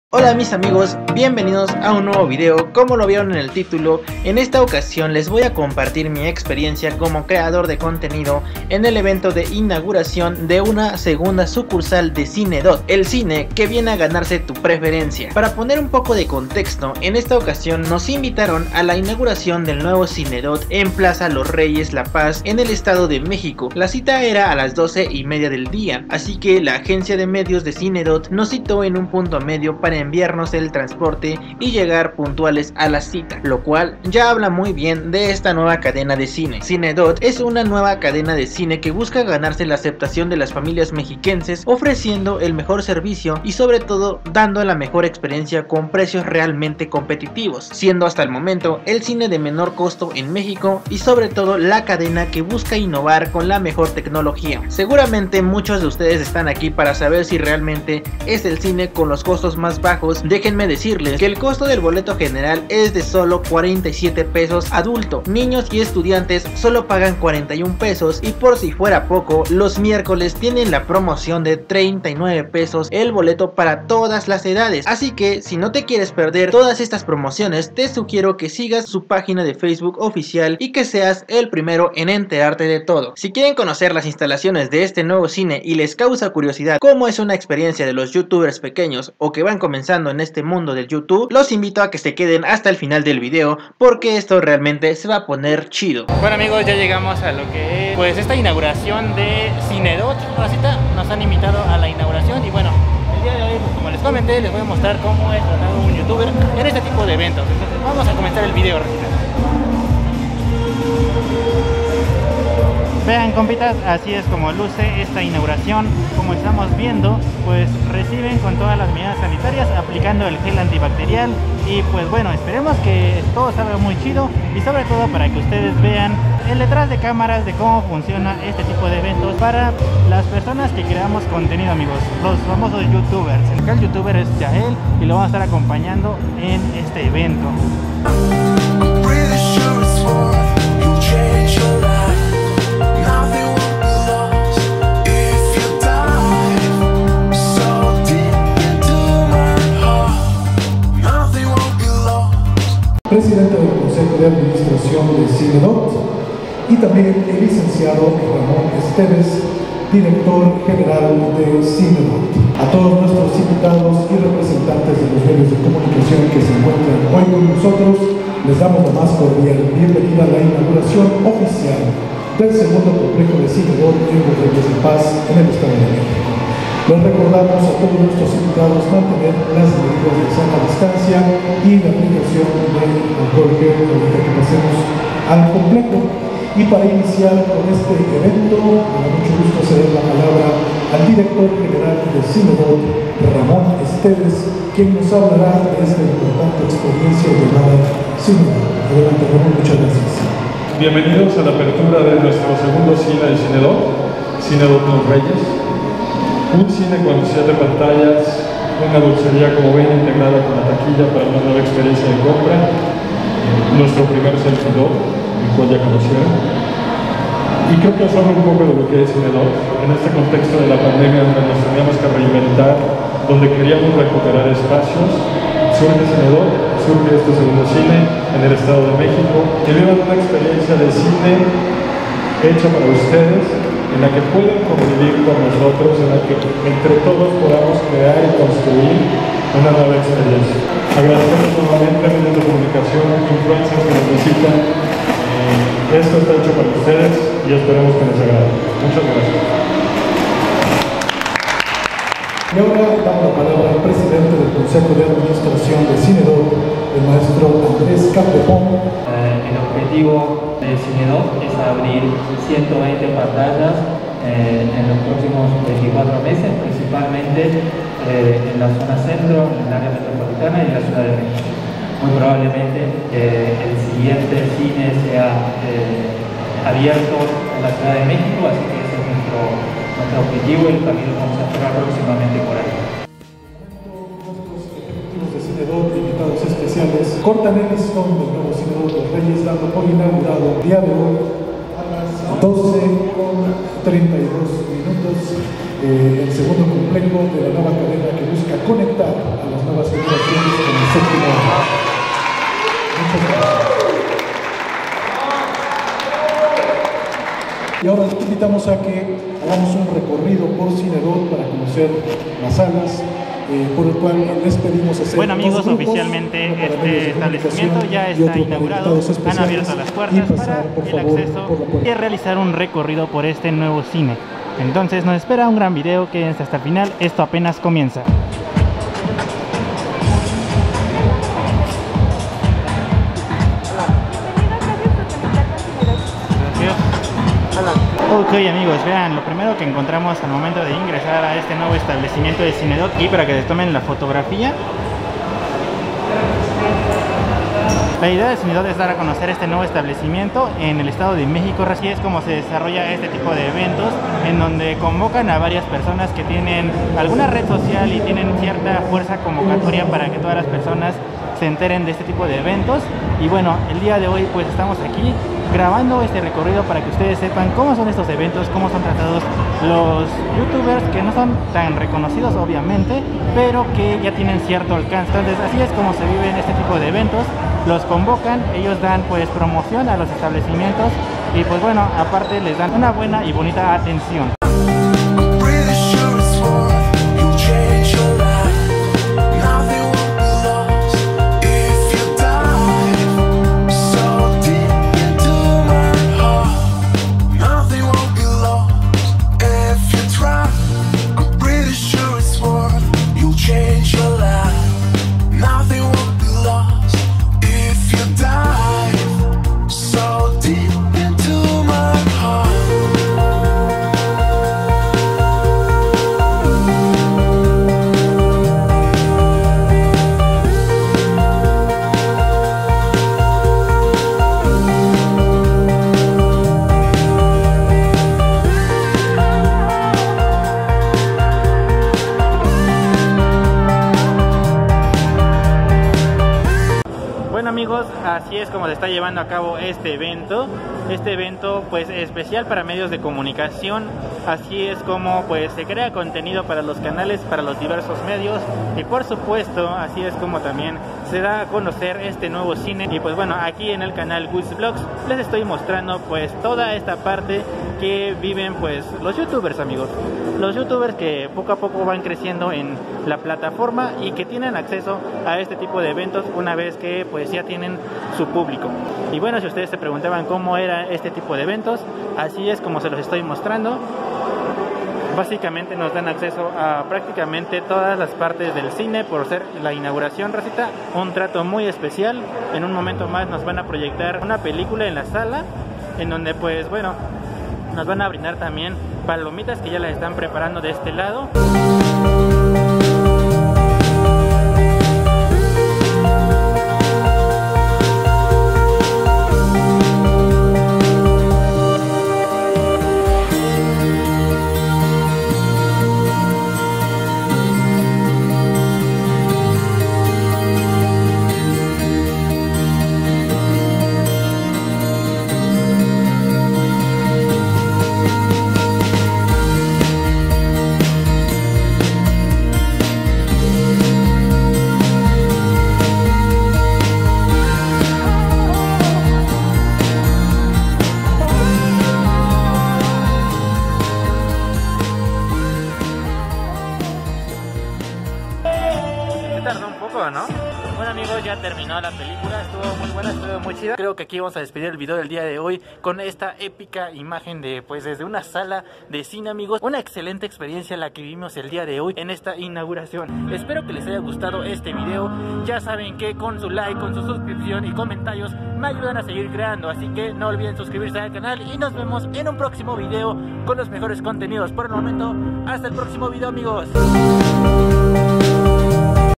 ASÍ TRATAN A LOS YOUTUBERS EN ESTOS EVENTOS! Cinedot Inauguración Plaza de La Tecnología-Los reyes
Fui a la inauguración de la cadena de Cine Cine Dot en Plaza de la tecnología los reyes la paz en el estado de Méxcio y así estuvo.
¿Cómo son los eventos de invitación de Youtubers pequeños? Inauguración CINE DOT Plaza Los Reyes Edo. México.
Hola mis amigos bienvenidos a un nuevo video como los vieron en el titulo del video en esta ocasión les voy a compartir mi experiencia como creador de contenido en el evento de inauguración de una segunda sucursal de CINE DOT el Cine que viene a ganarse tu preferencia.
Para poner un poco de contexto, en esta ocasión nos invitaron a la Inauguración del nuevo CINE DOT en plaza los Reyes la paz en el Estado de México, la cita era a las 12:30 del día en plaza de la tecnología, así que la agencia de medios de CINE DOT nos citó en un punto medio para enviarnos el transporte y llegar puntuales a la cita, lo cual ya habla muy bien de esta nueva cadena de cine.
Cine DOT es una nueva cadena de cine que busca ganarse la aceptación de las familias mexiquenses ofreciendo el mejor servicio y sobre todo dando la mejor experiencia con precios realmente competitivos, siendo hasta el momento el cine de menor costo en México y sobre todo la cadena que busca innovar con la mejor tecnología.
Seguramente muchos están aquí para saber si realmente es el cine con los costos más bajos y para que tengan el dato a la voz de ya.
Déjenme decirles que el costo del boleto general es de solo $47 adulto, niños y estudiantes solo pagan $41 y por si fuera poco los miércoles tienen la promoción de $39 el boleto para todas las edades, así que si no te quieres perder todas estas promociones te sugiero que sigas su página de Facebook oficial y seas el primero en enterarte de todo.
Les recuerdo que esta es la segunda sucursal de esta cadena, la primera está en plaza Coacalco , pero por supuesto la visión de la cadena es en un futuro tener presencia en distintas ciudades del país.
Si quieren conocer las instalaciones de este nuevo cine y les causa curiosidad como es una experiencia de los youtubers pequeños y este mundo de los medios de comunicación los invito a que se queden hasta el final del video porque esto realmente se va poner chido.
Bueno amigos realmente me siento muy feliz de estar en mis primeros eventos como medio de comunicación y porque no decirlo haciendo el papel de creador de contenido, desde hace algunos días he querido hacer un video de este tipo abordando este tema, ya que en lo personal antes de comenzar con el canal tenia la curiosidad de como era el trato que se les daba a los youtubers pequeños por decirlo así, debido a que claramente en mi caso apenas vamos incursionando en este mundo y aun no tengo una gran audiencia, sin embargo, se que con su apoyo poco a poco lo iremos logrando.
Pero en fin en esta ocasión tenemos la oportunidad de estar en la inauguración de la cadena de cine Dot que seguramente muchas personas quisieran que llegara a sus ciudades, y por supuesto te invito a que dejes en los comentarios de que parte de México miras este video y si quisieras que esta cadena llegara a tu ciudad.
Terminando el acto simbólico de corte de listo nos dieron la libertad de recorrer las salas de Cine dot en compañía de su personal de medios y sus fundadores. Caminamos por sus pasillos y todo se veían en excelentes condiciones. Para finalizar el evento Cine Dot nos proyecto uno de sus estrenos del día que fue la película de Reminiscencia, la función tuvo lugar en la sala 1 acompañado de un combo completo de palomitas y bebidas. Así es como estábamos cerrando el día en esta inauguración que en lo personal me la pase increíble.